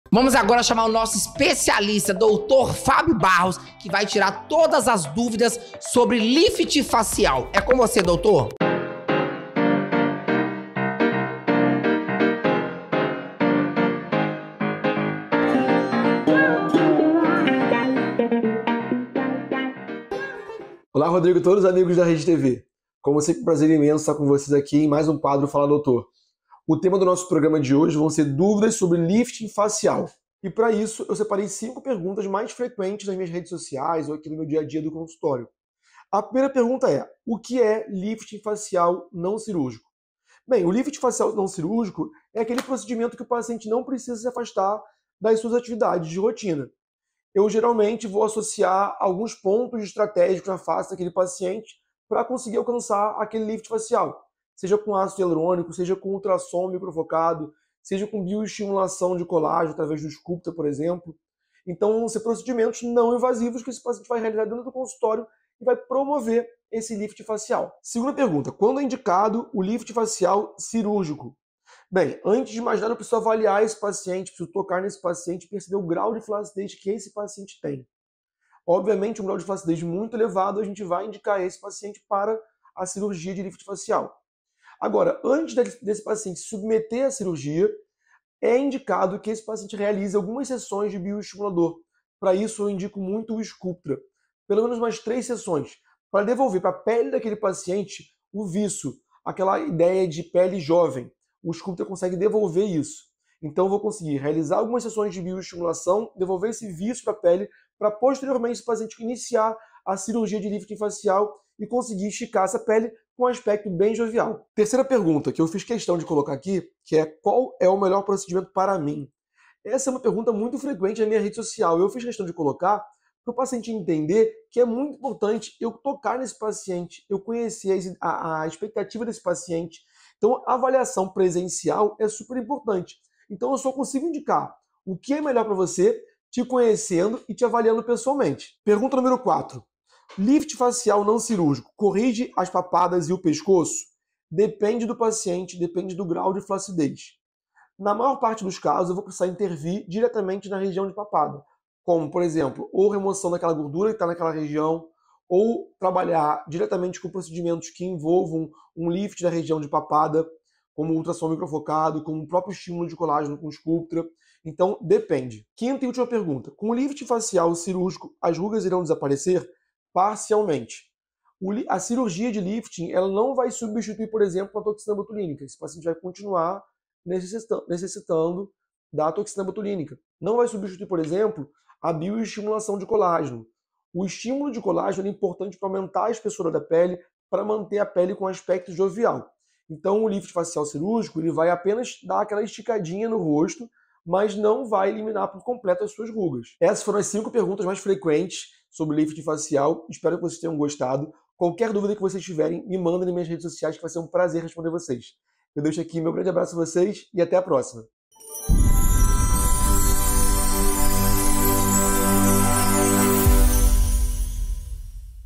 Dúvidas sobre Lifting Facial - PGM 077
Dr. Fabio Barros tirou algumas dúvidas sobre Lifting Facial.